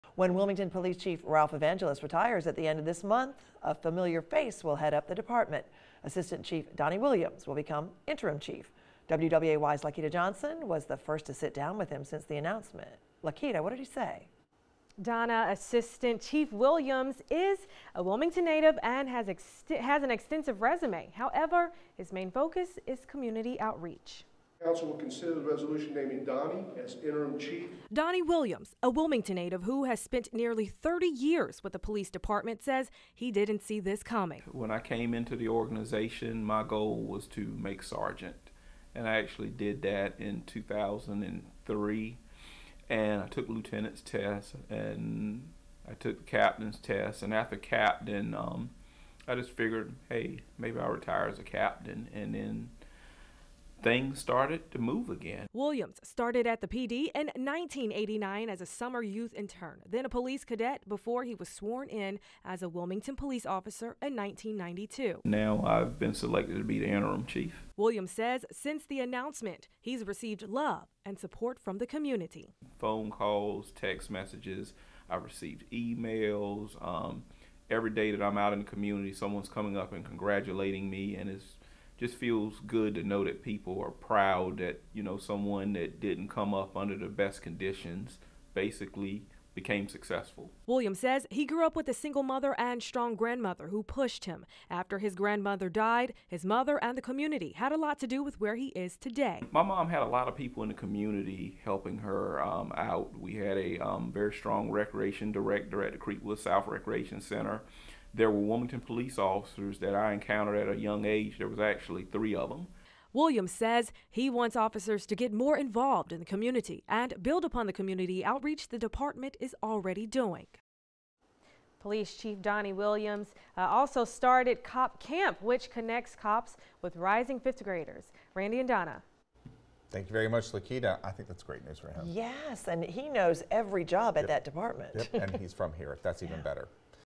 Wilmington Police Department
Wilmington PD gets interim police chief.